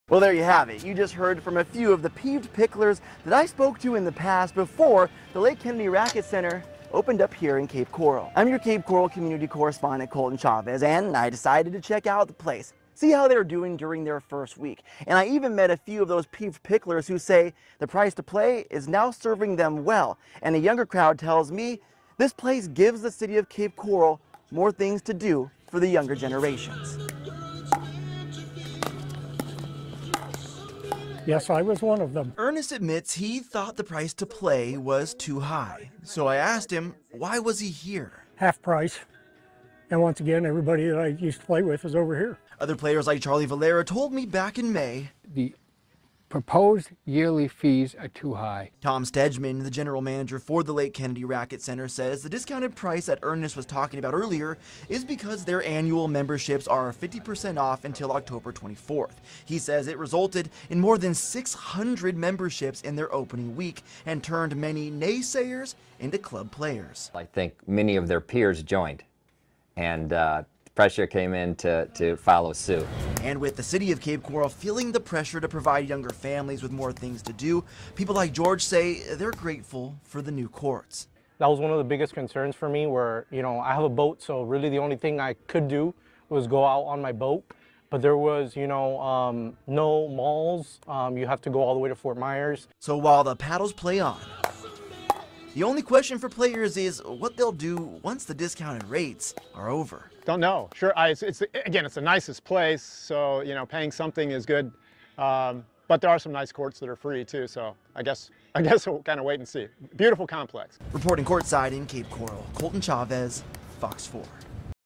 New pickleball center does big numbers despite previous pushback in Cape Coral
On Friday, pickleball players like Ernest said he used to be one of the players who said the price to play at Lake Kennedy Racquet Center was too high.